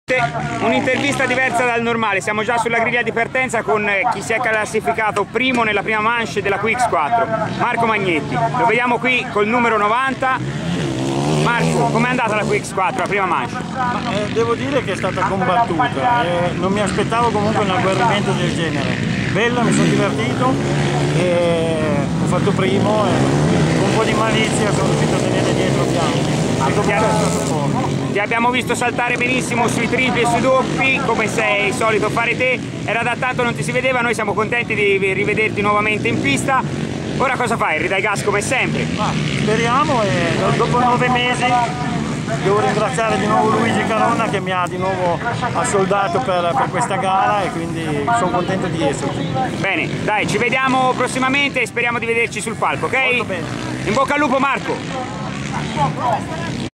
2011_09_18_gara2_linea_magnetti.MPG
2011_09_18 campionato italiano quad Cross FMI, videointervista sulla linea di partenza a Marco Magnetti, 6° tappa, crossodromo di Borgo Santa Rita - Grosseto, diritti di Quaddisti.it, pubblicazione videoarticolo completo su Radio box di Quaddisti.it,